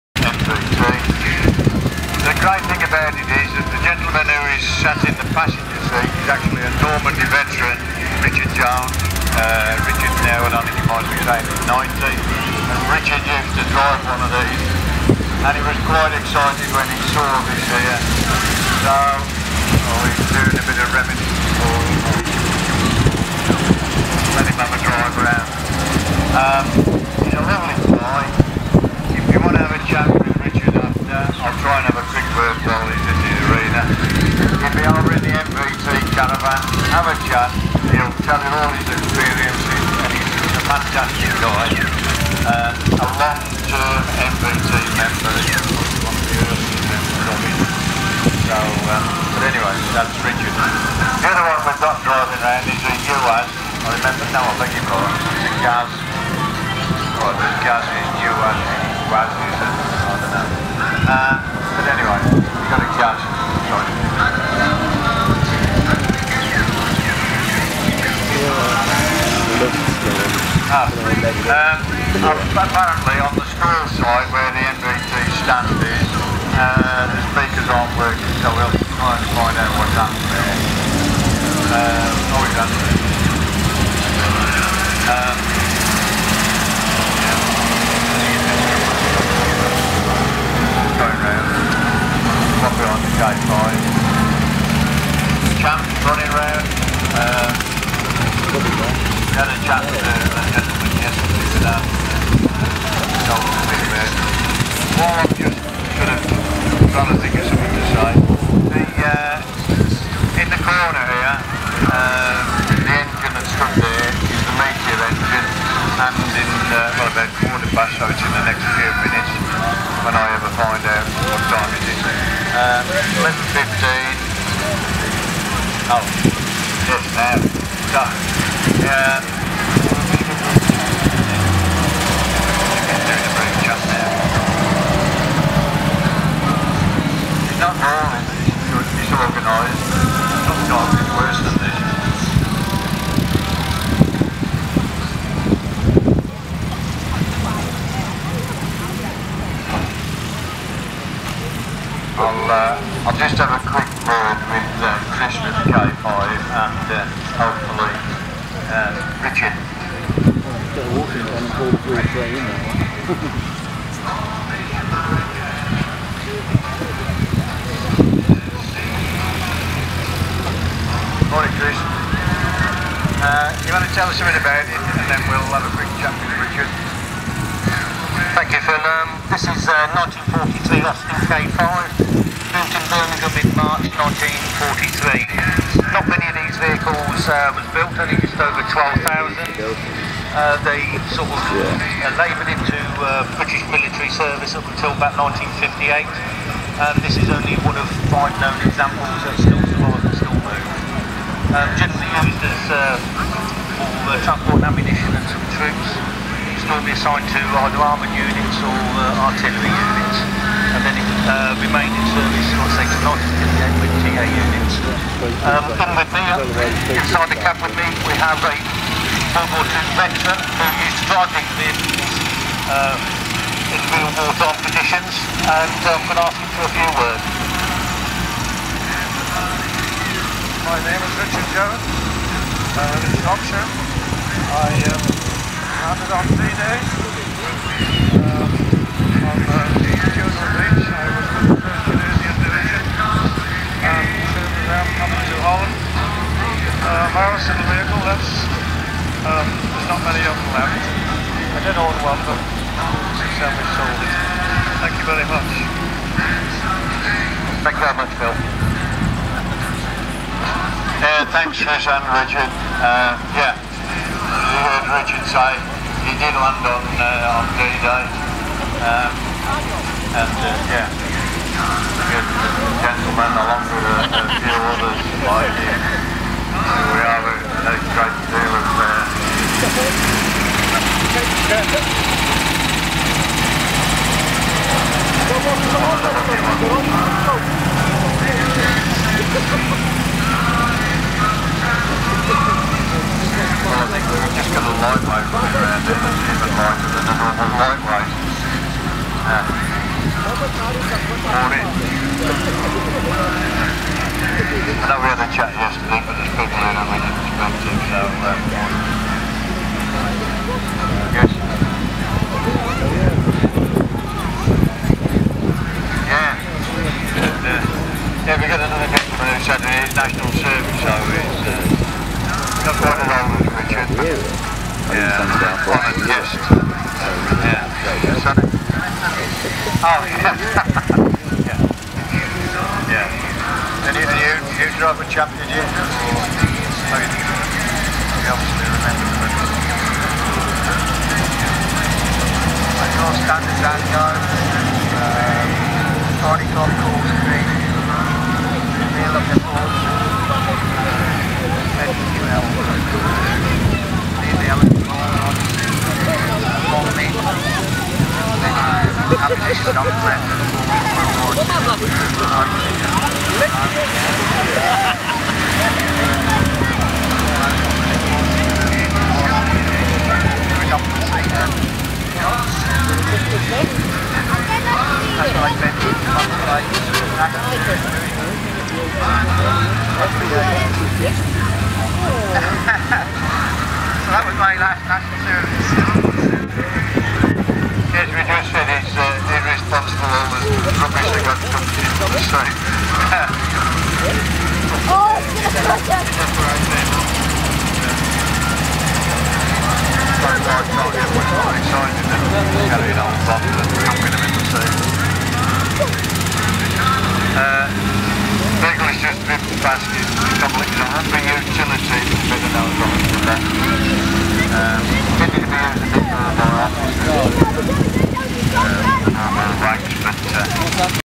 Tanks Trucks And Firepower Show 2013 Soft Skin Vehicle Display
Vehicles Featured are :
1943 Austin K5 PPM 319
1954 Austin Champ NPU 56J
1954 Austin Champ KNX 439D
1969 Land Rover Series 2A TRE 336 R
1971 FAZ 69-68E Orel-1 DDG 970J
1943 Humber Heavy Utility 631 YUP